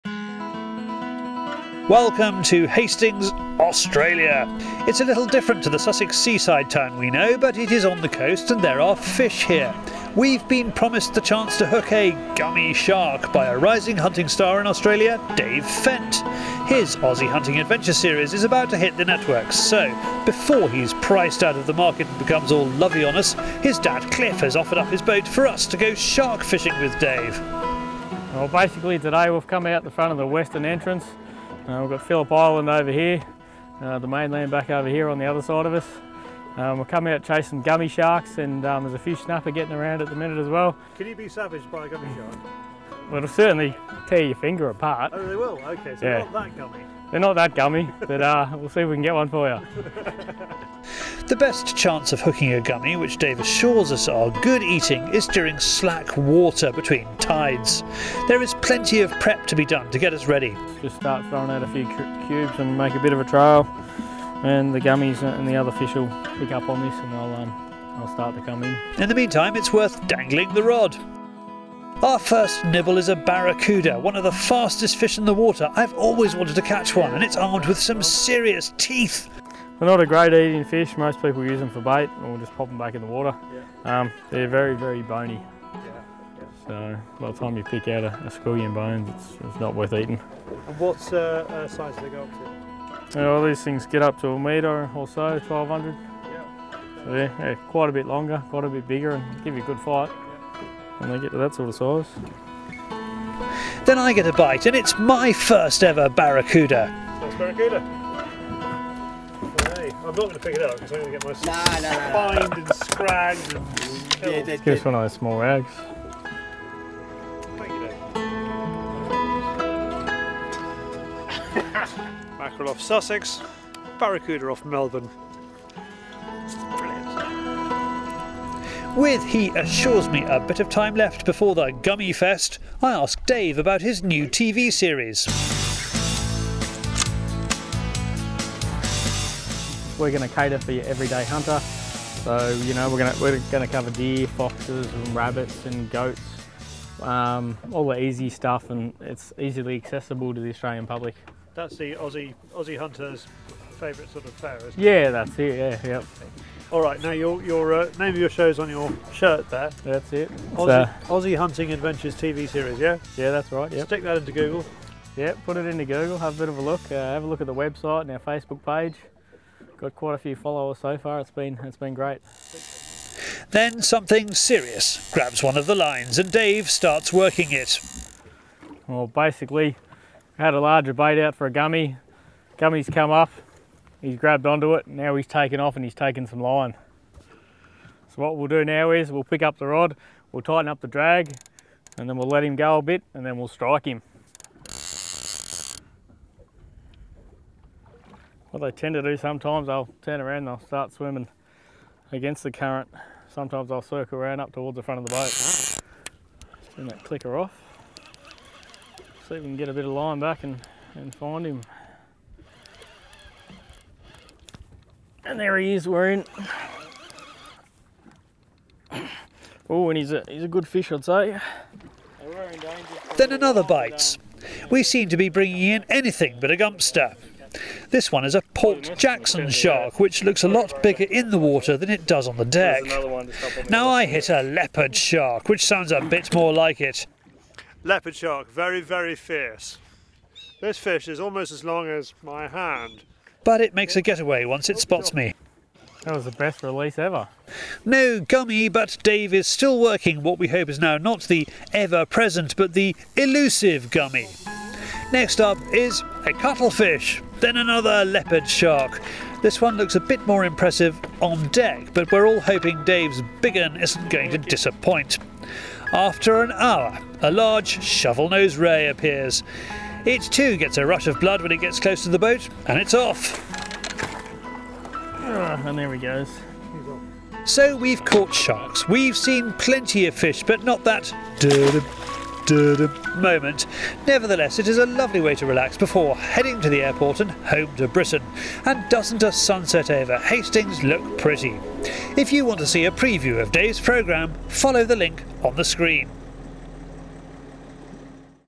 Fishing in Australia - barracuda and gummy sharks
We're out fishing off Hastings, Victoria, in a beatifully equipped sea boat, and in the company of Dave Fent, who runs Aussie Hunting Adventures on Australian TV, and Dave's dad Cliff. We get into all kinds of shark, including leopard and Port Jackson shark. And Dave has a preview of some of his hunting TV shows coming out later in the year.

This item first appeared in Fieldsports Britain, episode 185.To watch the whole show go